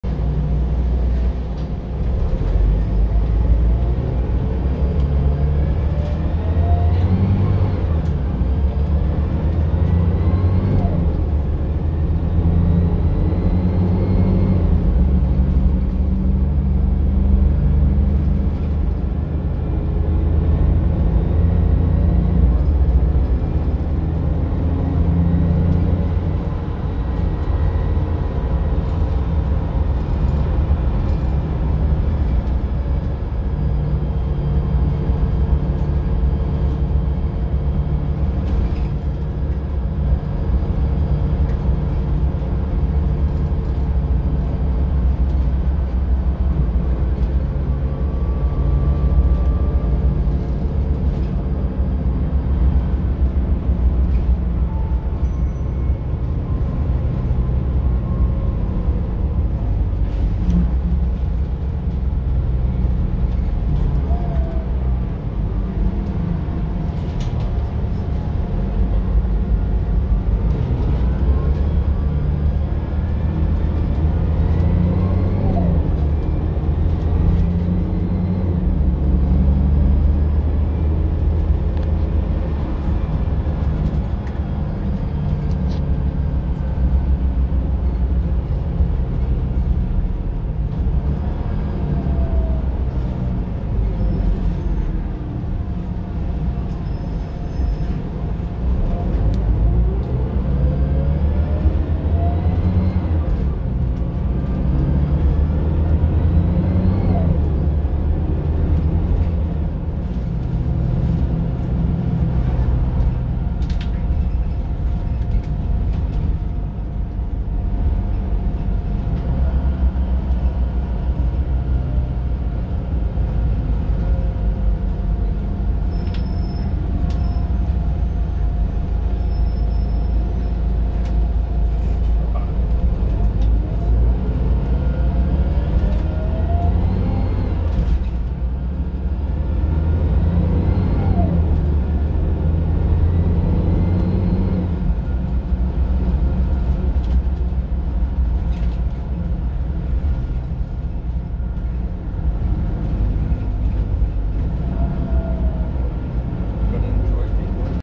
Transperth 1866 - Mercedes-Benz OC500LE CNG midi
Route: 68
Depot: withdrawn from service (Morley at time of filming)
Filmed with a number of young enthusiasts (who turned out to all be foamers).

Filmed: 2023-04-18, file size: 535,761,971 bytes